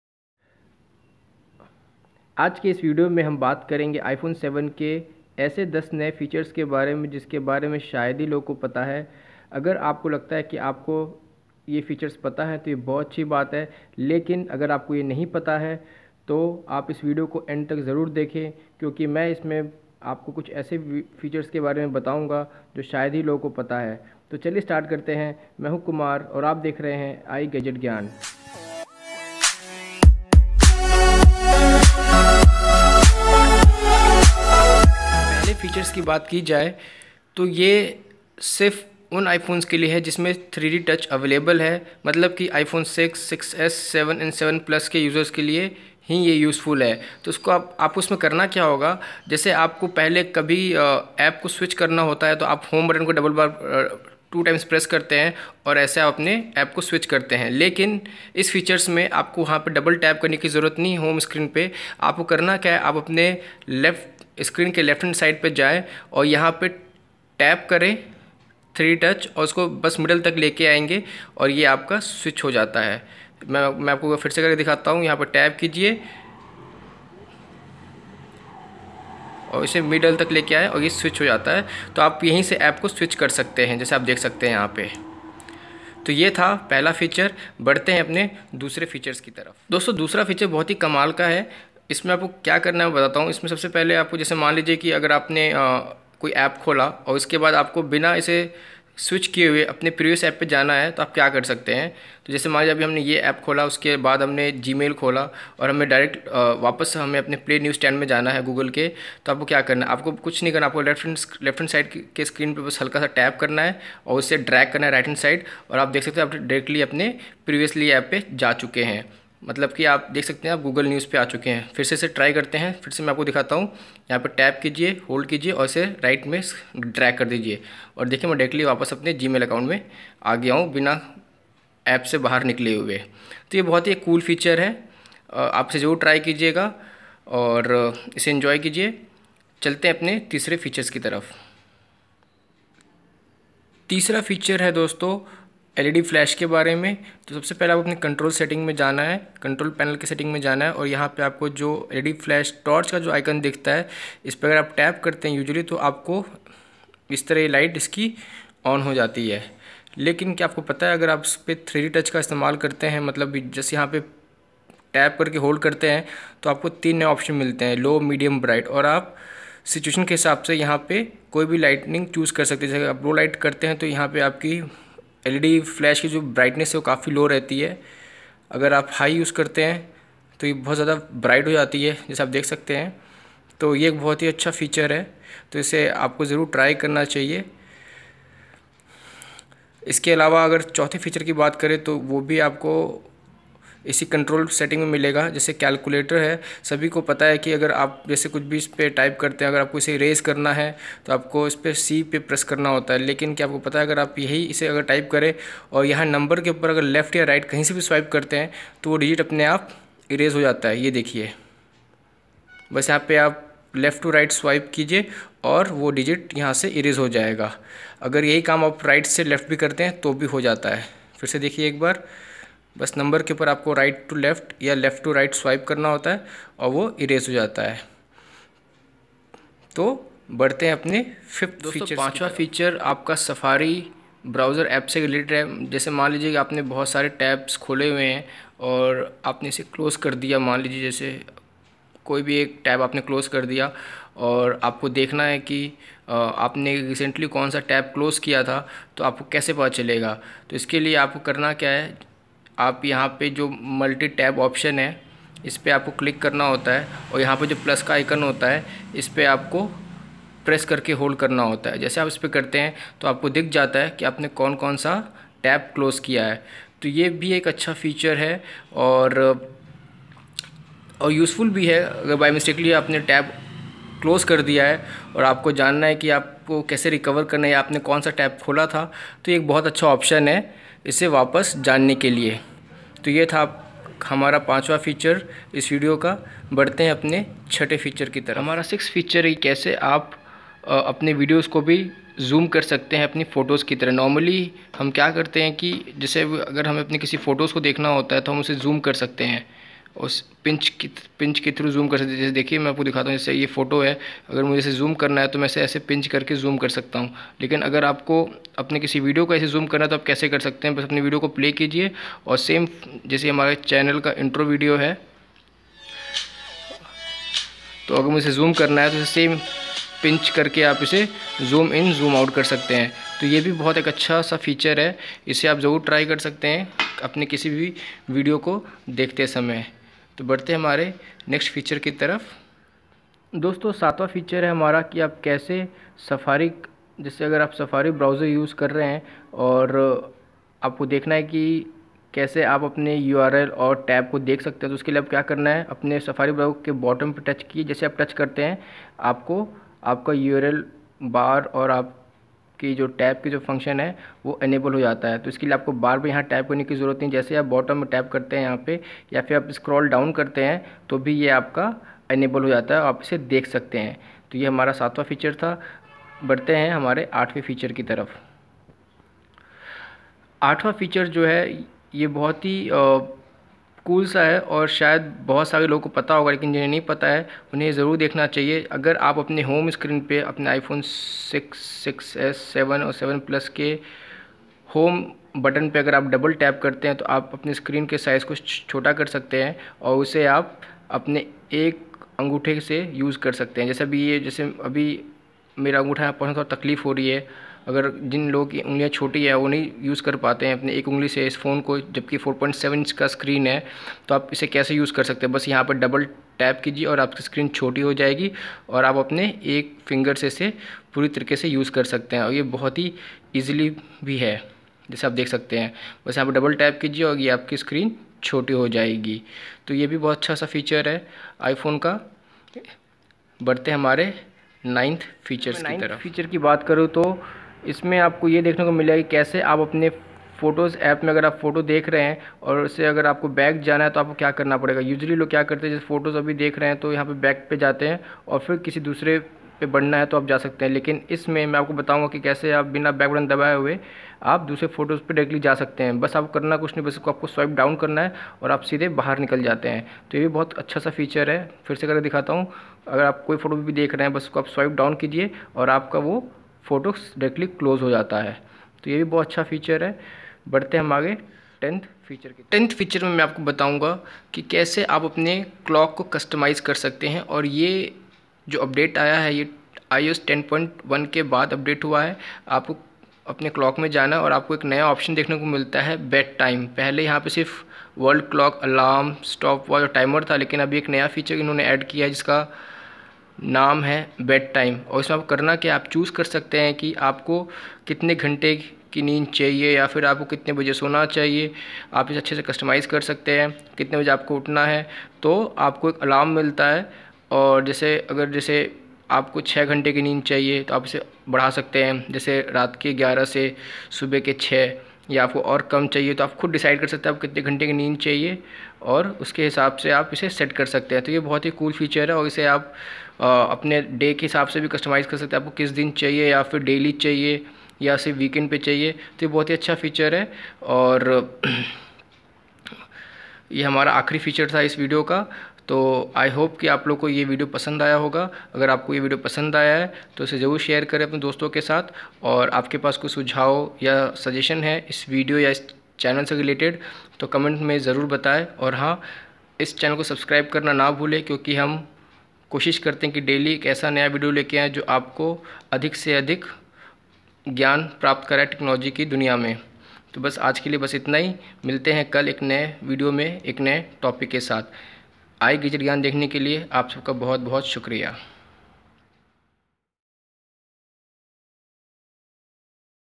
iPhone 7 Top 10 Hidden Features | iPhone 7 Review 2020 | iPhone 7 Tips & Tricks 2020
learn iPhone 7 top 10 new hidden features or gestures in this video.

नमस्कार दोस्तों

इस विडियो में मैंने iPhone 7 की 10 नयी बातों के बारे में बताया है की वह कैसे काम करता है ।

iGadget ज्ञान को देखने के लिए आपका बहुत शुक्रिया |